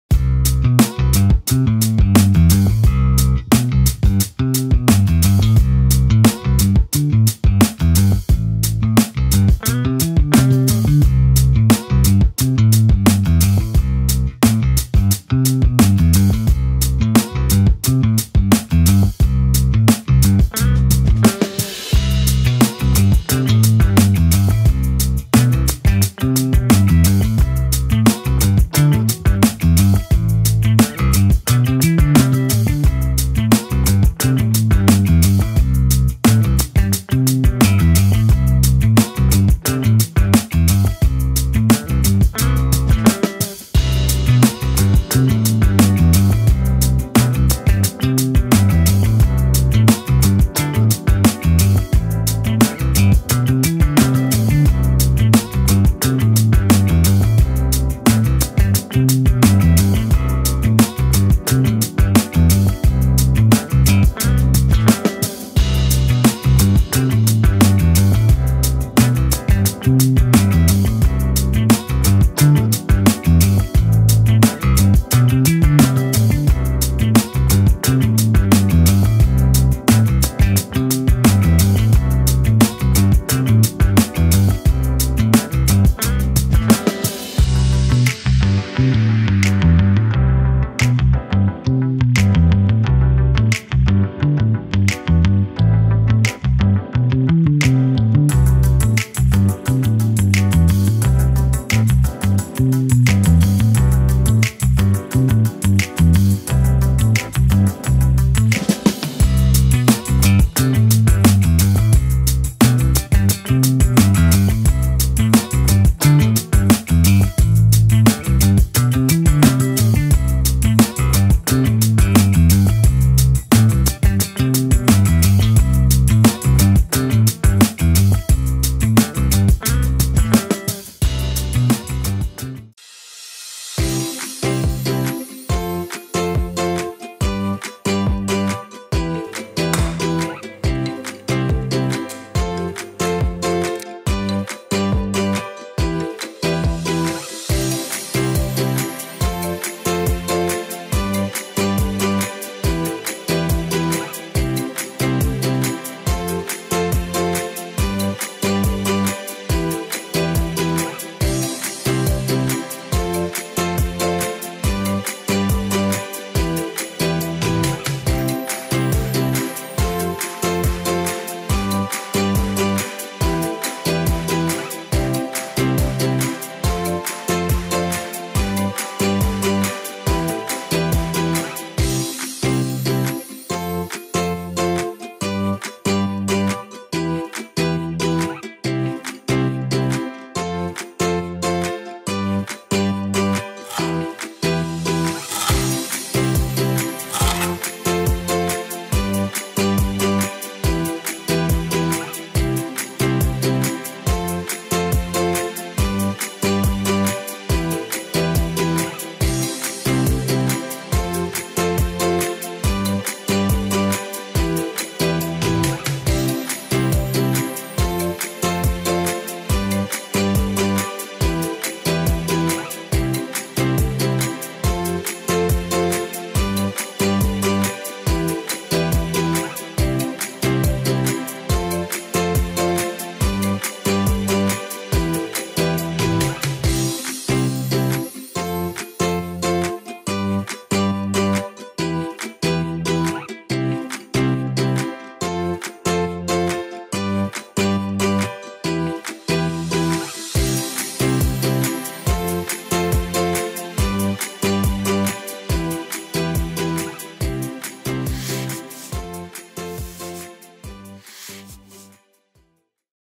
Ottimo trucco! Formaggio fatto in casa con latte e sale
Ingredienti:
latte - 4 l
acqua - 80 ml
aceto 9% - 6 cucchiai
sale - 20 g
lievito in polvere - 10 g
AL FREDDO 24 H